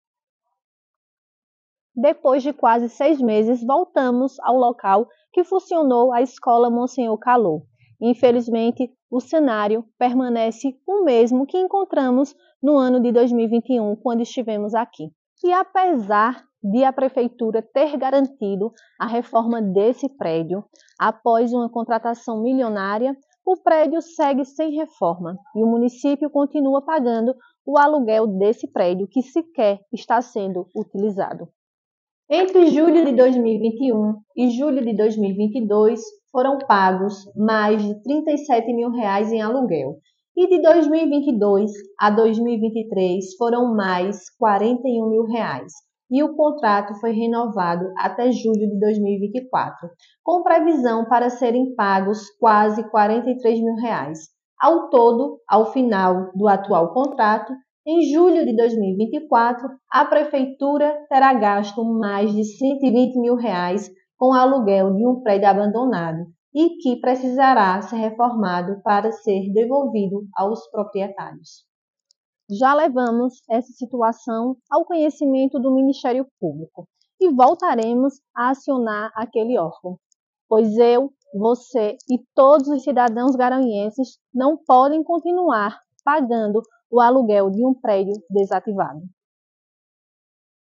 SECRETARIA DE EDUCAÇÃO SEGUE PAGANDO ALUGUEL DE PRÉDIO DE ESCOLA DESATIVADA DESDE 2021
“A Prefeitura de Garanhuns terá pago, até julho de 2024, mais de R$ 120 mil reais com o aluguel de um Imóvel que está abandonado pela Secretaria de Educação”. A informação foi registrada pela vereadora Magda Alves (PP), em contato com o Blog do Carlos Eugênio.

Segundo Magda, a Escola Municipal Monsenhor Callou, localizada na Praça Campos Sales, está desativada desde 2021 (relembre clicando AQUI), todavia, a Prefeitura vem pagando o aluguel do imóvel como se estivesse em pleno funcionamento.

“Entre julho de 2021 e julho de 2022 foram pagos mais de R$ 37 mil reais em aluguel. De 2022 a 2023 foram mais R$ 41 mil reais e o contrato foi renovado até julho de 2024, com previsão para serem pagos quase R$ 43 mil reais. Ao todo, ao final do atual contrato, a Prefeitura terá gasto quase R$ 121 mil reais com o aluguel de um prédio abandonado”, destacou Magda Alves, que registrou ter levado o assunto, com documentos comprobatórios, ao conhecimento do Ministério Público e do Tribunal de Contas do Estado, em agosto passado.

A POSIÇÃO DA PREFEITURA – Em agosto passado, a Secretaria Municipal de Educação registrou, em Nota enviada ao Blog do Carlos Eugênio, que o prédio da Escola seria reformado para posterior entrega a proprietária. É que segundo a Educação Municipal “o contrato determina que para o prédio ser devolvido ao proprietário, deve estar nas mesmas condições em que foi locado”. Passados quatro meses, o Blog do Carlos Eugênio buscou uma posição atualizada da Secretaria, porém ainda não obtivemos retorno.

14/12/2023